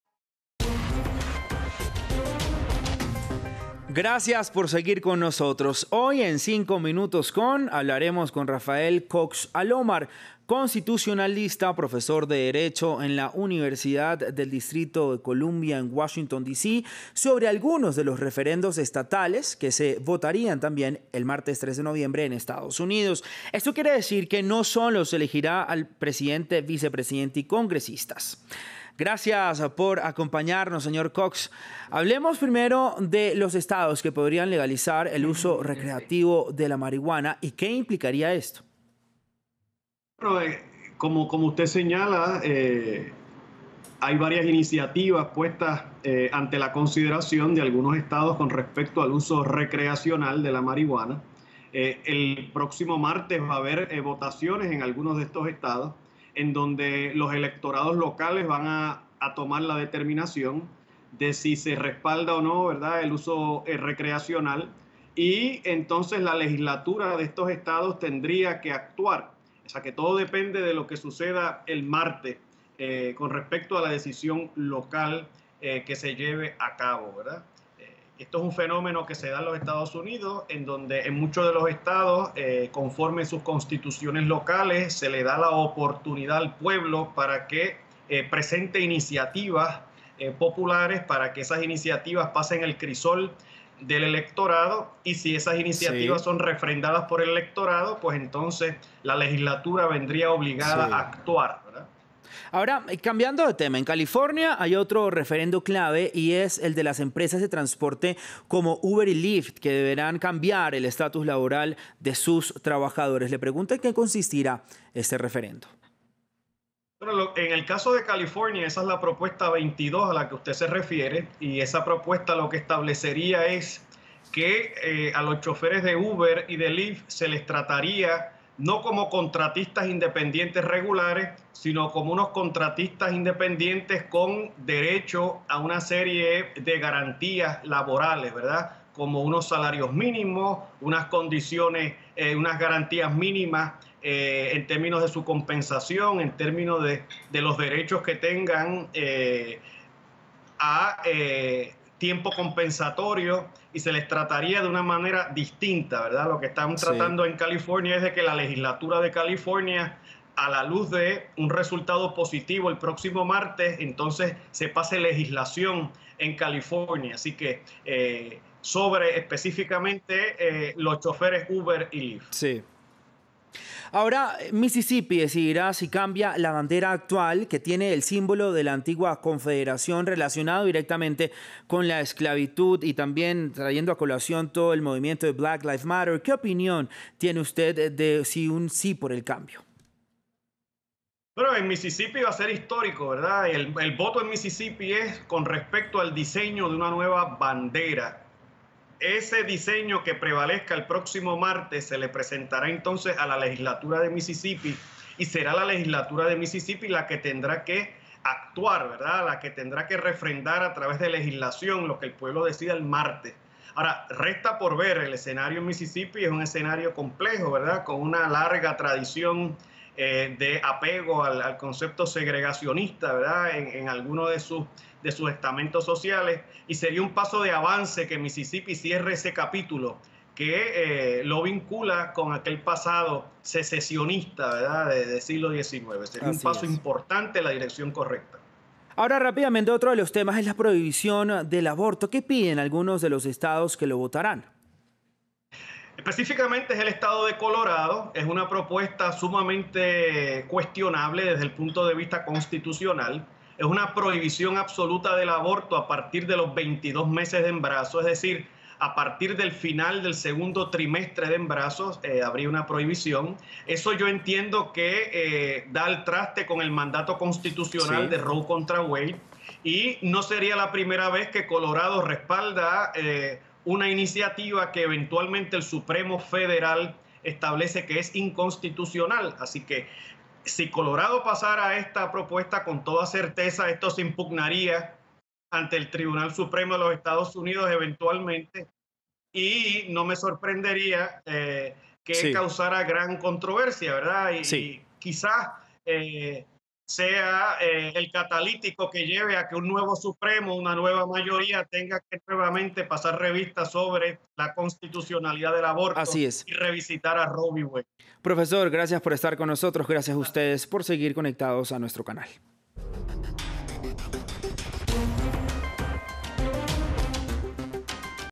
Rafael Cox Alomar: "En EE. UU. el pueblo decide sobre iniciativas populares a través de referendos"
Este 3 de noviembre los estadounidenses también votarán una serie de referendos estatales en los que decidirán diversos temas como la legalización del uso recreativo de la marihuana, la regulación laboral de trabajadores de aplicaciones como Uber y Lyft, cambiar la bandera de Mississippi para quitarle un símbolo de esclavitud y prohibir el aborto, entre otros. En esta edición de 5 minutos con… hablamos con Rafael Cox Alomar, constitucionalista y profesor de derecho en la Universidad de Columbia, sobre los detalles de algunos de estos referendos en Estados Unidos.